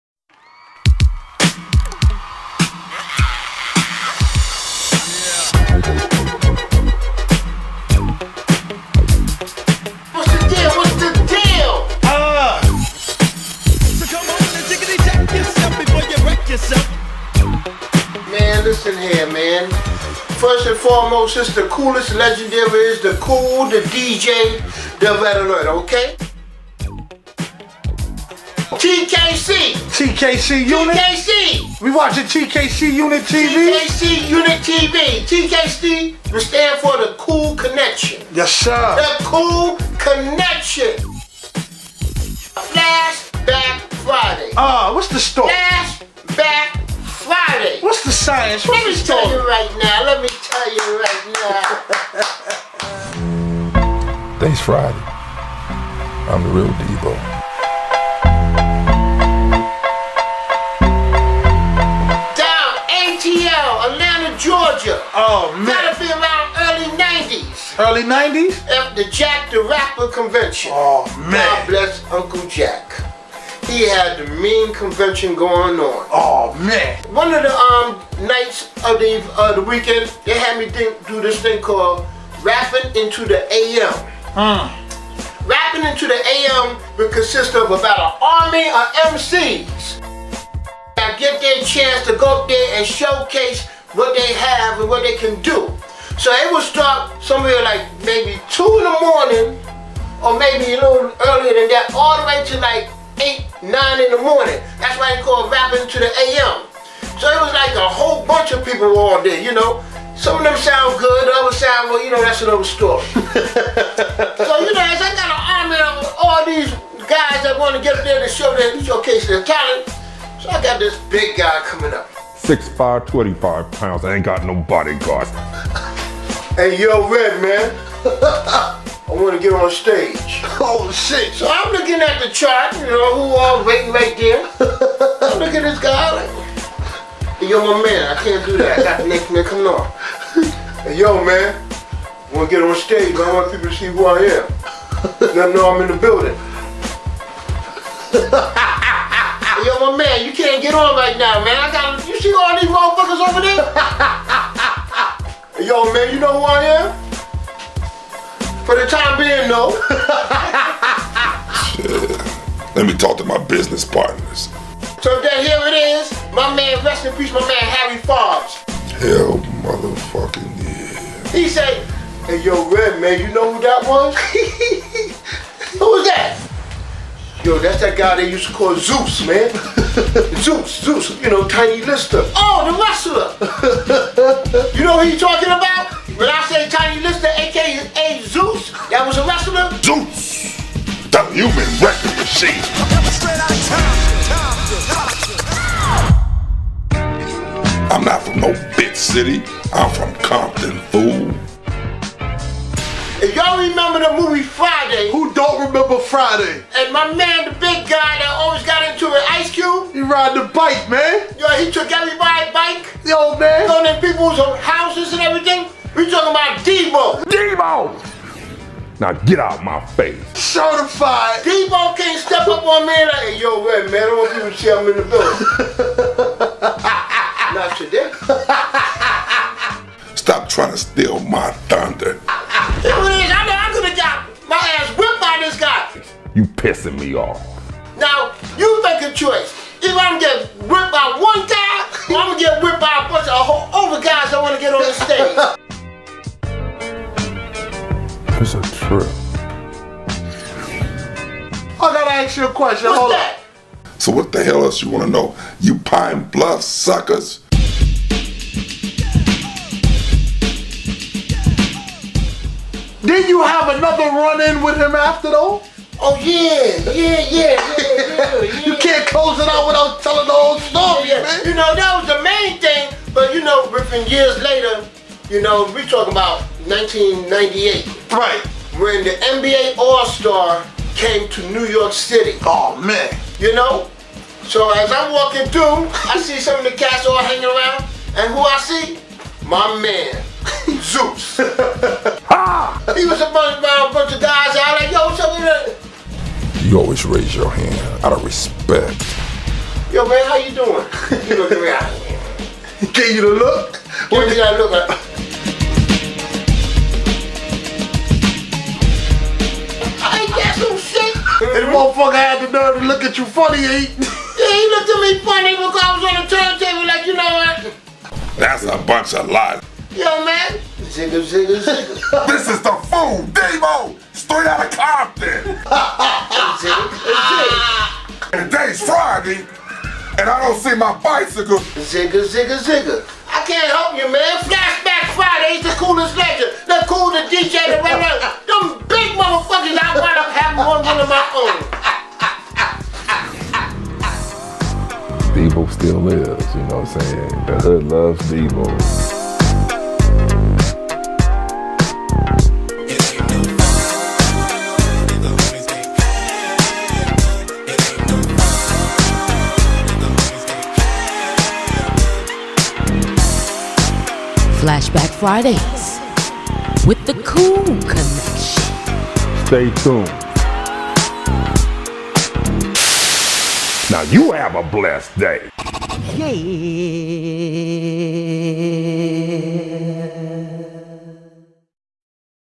TKC UNIT'S FLASHBACK FRIDAY-REMIX PT 23
CHECK OUT THE CLASSIC BUT FUNNY MOMENT BETWEEN TINY "ZEUS" LISTER & THE KOOLEST LEGEND KOOL DJ RED ALERT!

FOLLOW THE KOOLEST LEGEND ON TWITTER @KoolestLegend!